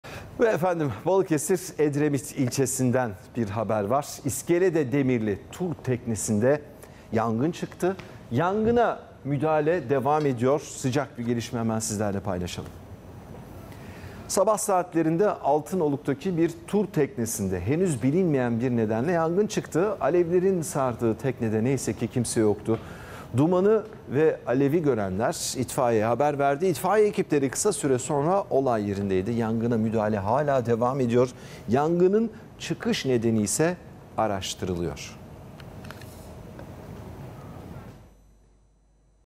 Altınoluk'ta tur teknesi alev alev yandı
Balıkesir'in Edremit ilçesine bağlı turistik Altınoluk Mahallesi'nde, iskelede bağlı tur teknesinde çıkan yangın, itfaiye ekiplerinin müdahalesiyle söndürüldü.

Altınoluk'ta iskeleye bağlı olan, 35 metre uzunluğundaki 'Ergün Ağa' isimli tur teknesinde, saat 06.30 sıralarında yangın çıktı. Elektrik panosundan kaynaklandığı sanılan yangında alevler, kısa sürede büyüdü ve teknenin yer yerini sardı. Sahildekiler ise kısa süreli panik yaşadı. İhbarla gelen itfaiye ekiplerinin müdahalesiyle söndürülen yangında tekne, kullanılamaz hale geldi.

Tur teknesinin makinisti, "Ben ve diğer personel teknede yatıyorduk. Dumanlar, sonra da alevler yükselmeye başlamış. Bizim personel fark etmiş. Hemen tekneden çıktı. Bu sırada yangın büyüdü. Elektrik panosundan çıktığını tahmin ediyoruz" dedi.